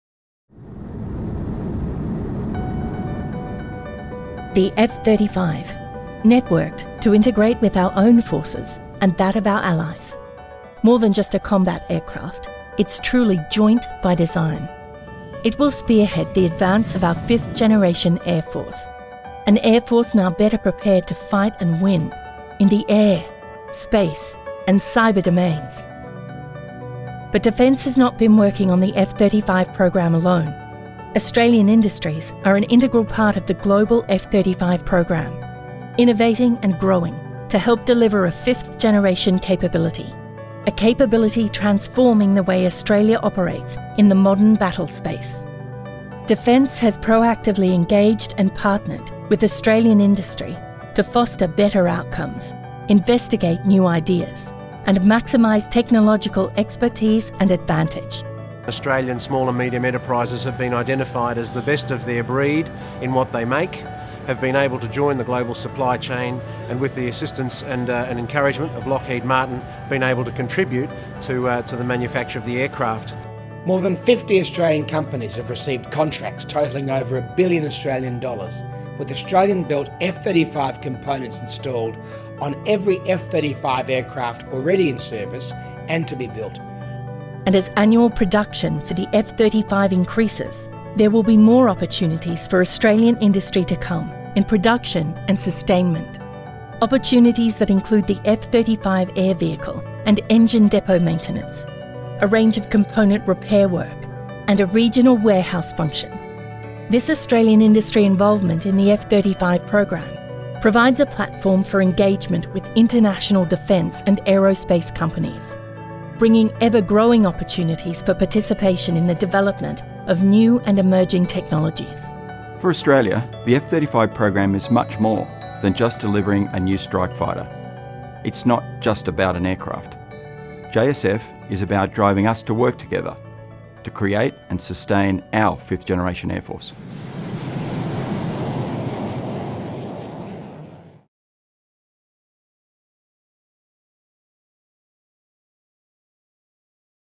Australian industry participation in the F-35 Joint Strike Fighter Program
The F-35A Joint Strike Fighter will deliver a fifth-generation capability that transforms the way we operate in the modern battlespace. The Program also delivers opportunities for Australian industry.

Did you know that there are Australian-built components on every F-35 aircraft produced to date? The cumulative total of Australian F-35 production contracts to date has reached over AU$1.2 billion. Over 50 Australian companies have shared in this success, with more than 2,400 jobs created.

Production volume for the F-35 will continue to increase, with Australian industry involvement expected to exceed AU$2 billion by 2023.

Watch this space for more videos on the F-35 Program and Australian industry participation.